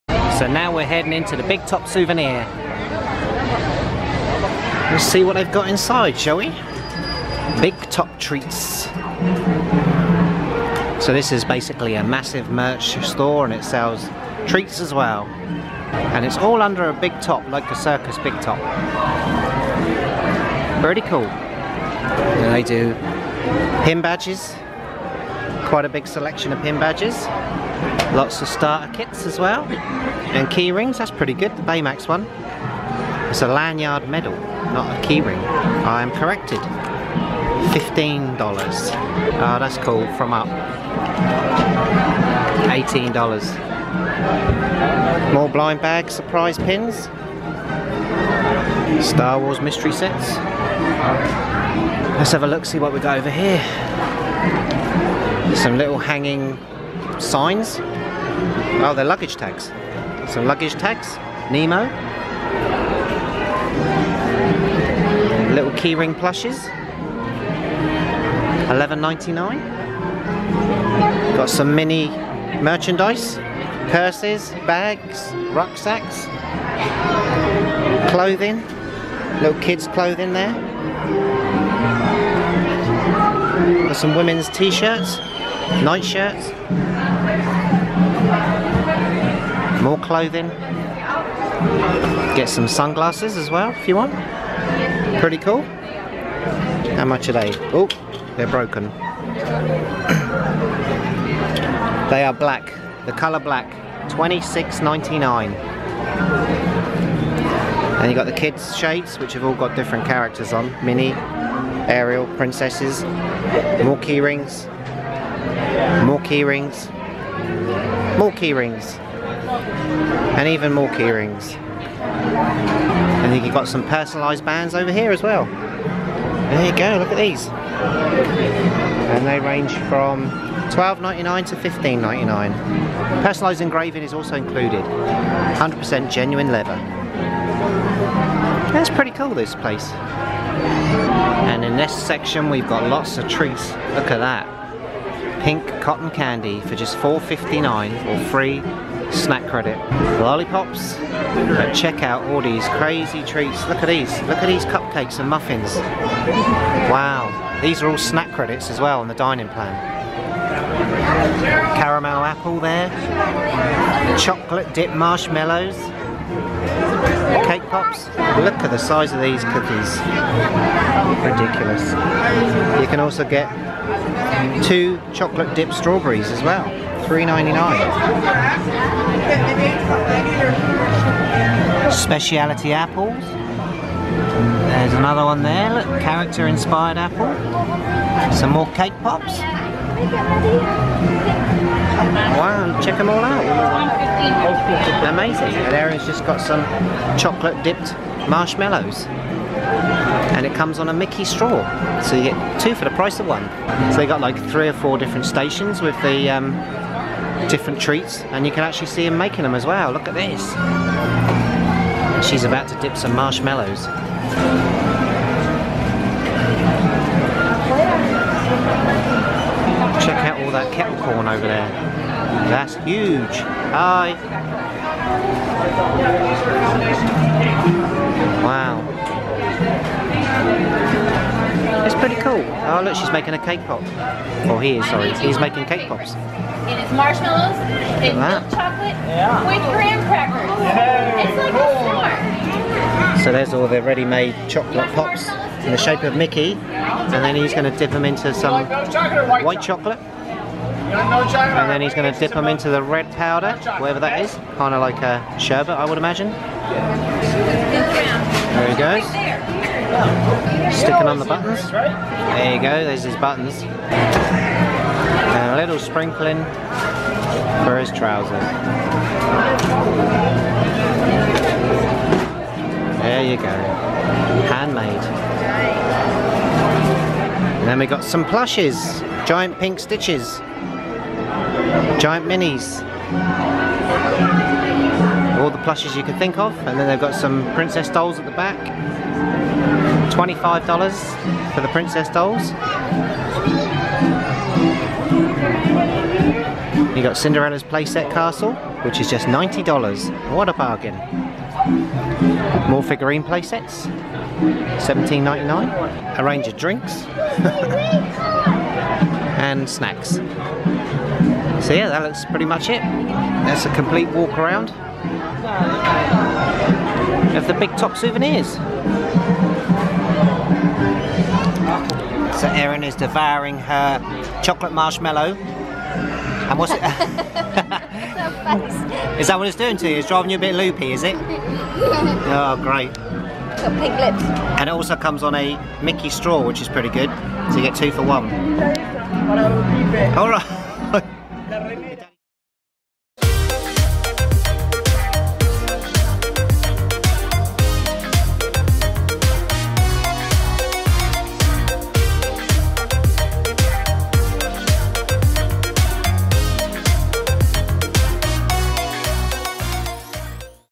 SHOPPING AT - BIG TOP SOUVENIRS - MAGIC KINGDOM - DISNEY WORLD
In this shopping vlog, we check out Big Top Souvenirs at Magic Kingdom, Walt Disney World.

Come join us... 'Living the Dream' 😀

🎧MUSIC 🎧